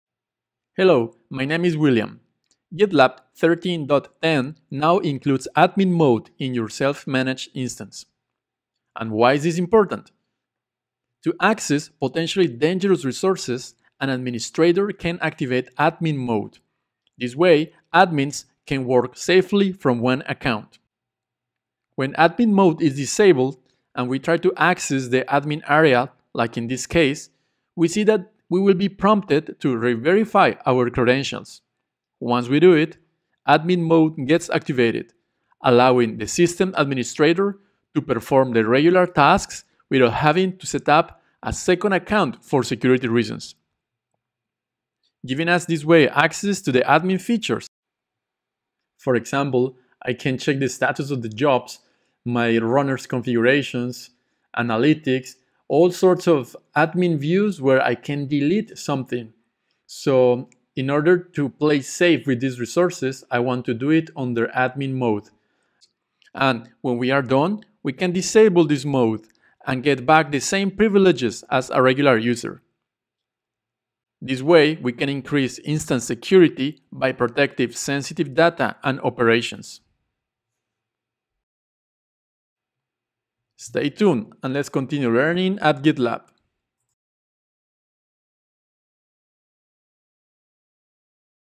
GitLab 13.10: Admin Mode: Re-authenticate for GitLab administration
GitLab now includes Admin Mode, which helps admins work safely from one account. When Admin Mode is not active, admins have the same privileges as a regular user. Before running administrative commands, admin users must reverify their credentials. Admin mode increases instance security by protecting sensitive operations and data. This feature is available behind feature flag :user_mode_in_session